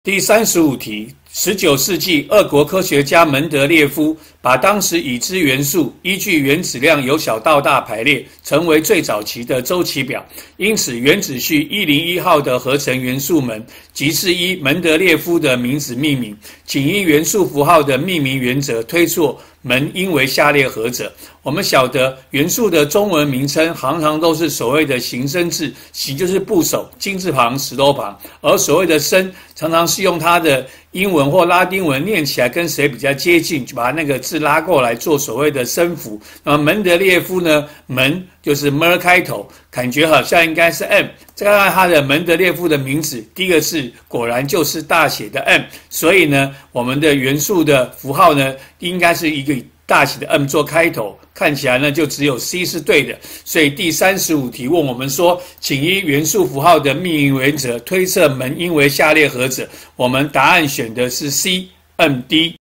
108學年上8年級理化三段第35題
問紀念門得列夫的元素「鍆」的元素符號。